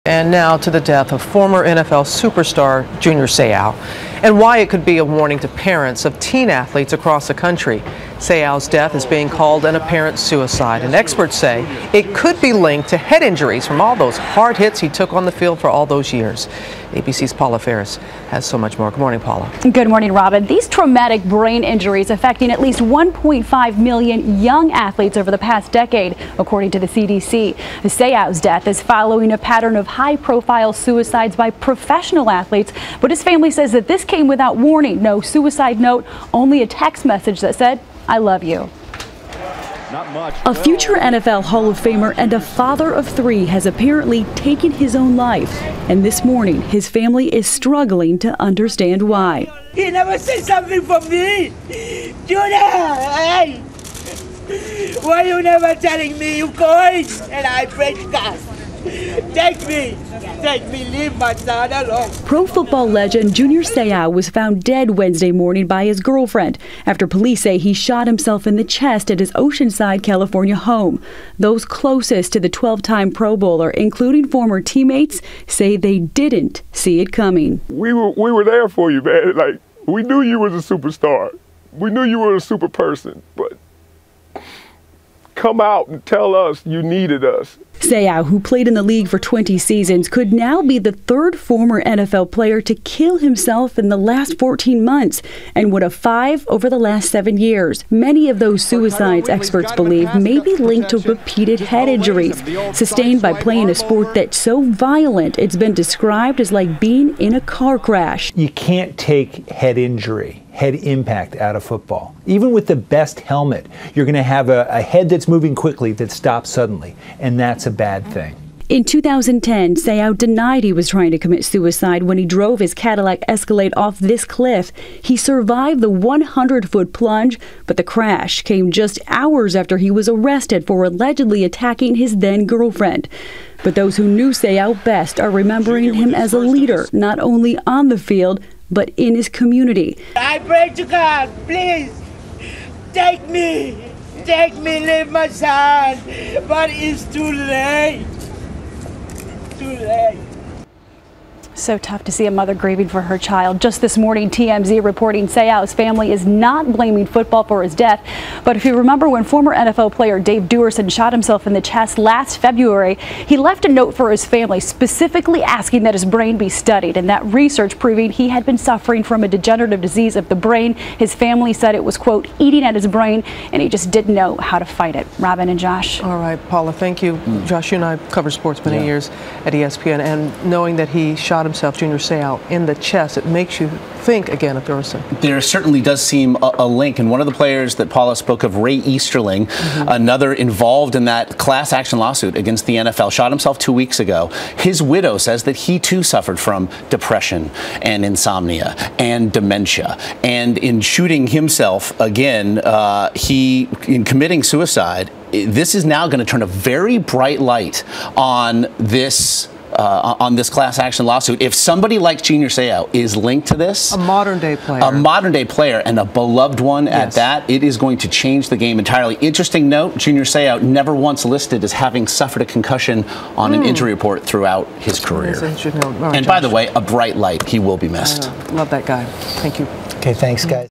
Junior Seau Found Dead; Mother Breaks Down After Ex-Charger, NFL Star Allegedly Shoots Self in Chest
Experts say the football player's head injuries may be linked to his death.